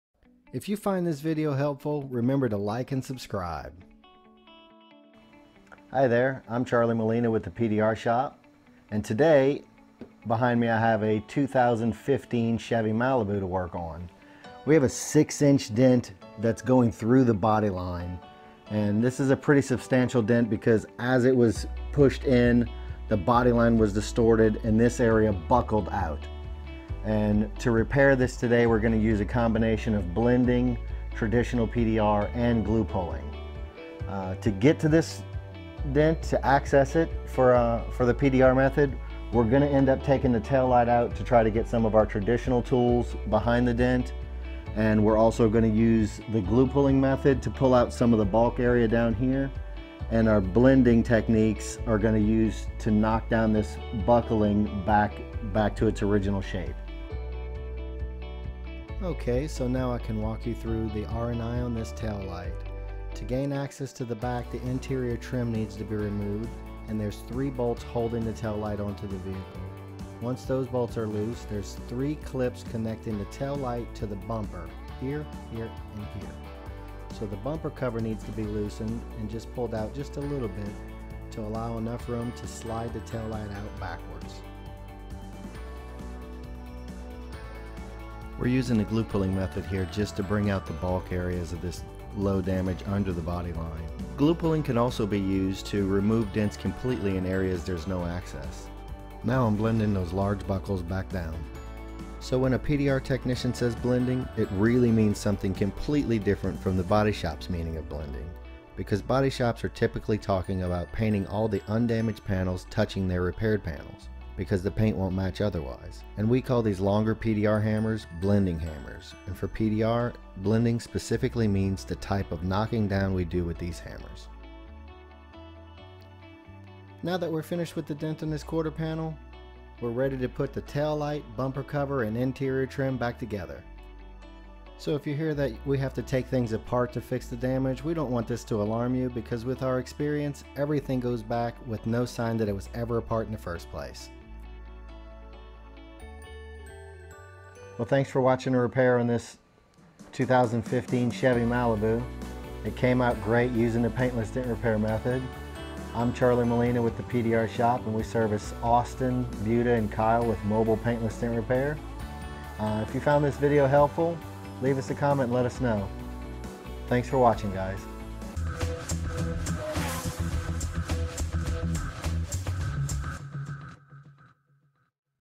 15 Chevy Malibu Large BodyLine Dent Repaired with Paintless Dent Repair Kyle TX, Buda TX, Austin TX
In this video we repair a large dent through the bodyline in the quarter panel of a 2015 Chevy Malibu using Blening, PDR Tooling, and Glue Pulling Paintless Dent Repair Techniques.

The PDR Shop offers mobile Paintless Dent Repiar to Kyle Tx, Buda TX, Austin TX, and the surrounding areas.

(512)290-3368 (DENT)